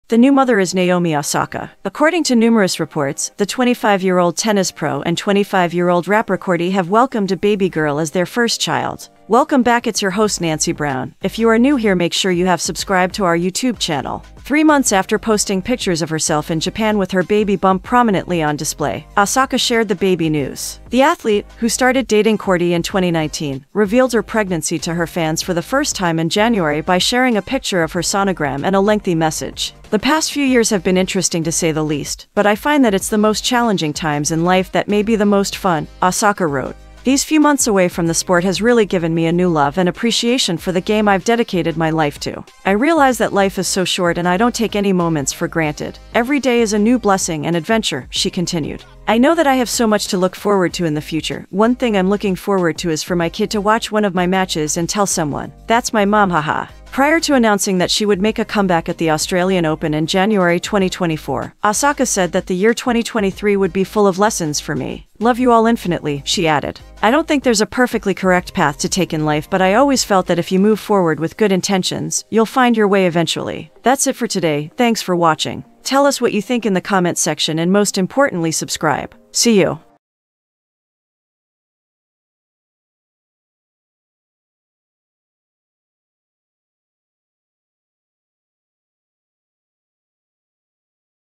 Naomi Osaka welcomes first baby with boyfriend Cordae
The new mother is Naomi Osaka! According to numerous reports, the 25-year-old tennis pro and 25-year-old rapper Cordae have welcomed a baby girl as their first child.
Three months after posting pictures of herself in Japan with her baby bump prominently on display, Osaka shared the baby news. The athlete, who started dating Cordae in 2019, revealed her pregnancy to her fans for the first time in January by sharing a picture of her sonogram and a lengthy message. “The past few years have been interesting to say the least, but I find that its the most challenging times in life that may be the most fun,” Osaka wrote. These few months away from the sport has really given me a new love and appreciation for the game I’ve dedicated my life to. I realize that life is so short and I don’t take any moments for granted, every day is a new blessing and adventure, she continued. I know that I have so much to look forward to in the future, one thing I’m looking forward to is for my kid to watch one of my matches and tell someone, ‘That’s my mom’ haha. Prior to announcing that she would make a comeback at the Australian Open in January 2024, Osaka said that the year 2023 would be "full of lessons for me." Love you all infinitely, she added.  I don’t think there’s a perfectly correct path to take in life but I always felt that if you move forward with good intentions, you’ll find your way eventually.